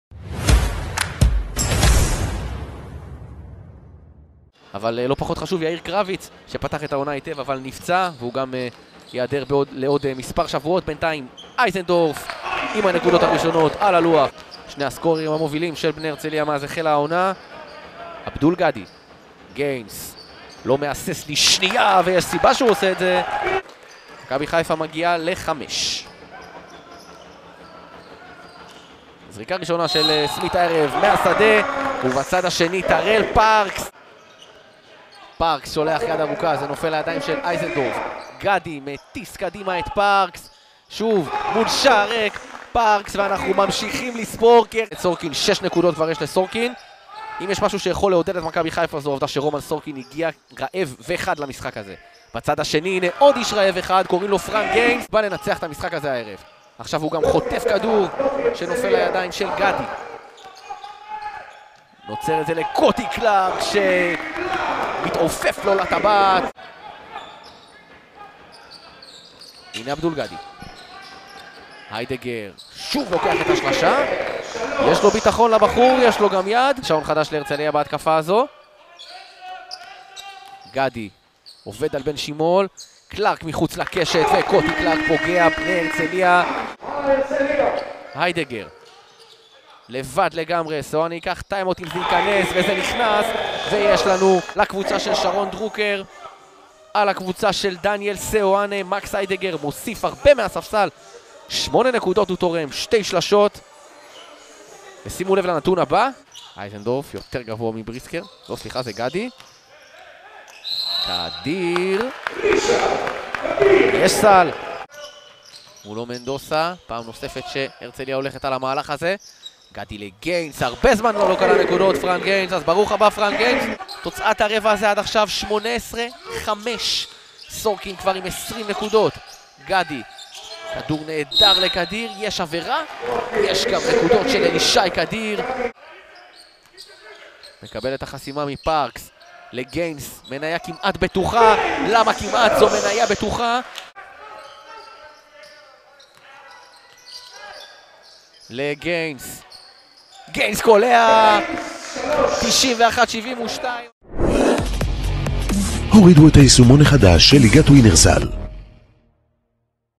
שיא מועדון לעבדול גאדי כבר בבכורה
הרכז החדש של בני הרצליה מסר לא פחות מ-16 אסיסטים והצטיין בניצחונה הראשון העונה של העולה החדשה. צפו בו מפנק את חבריו במסירות יוצאות מן הכלל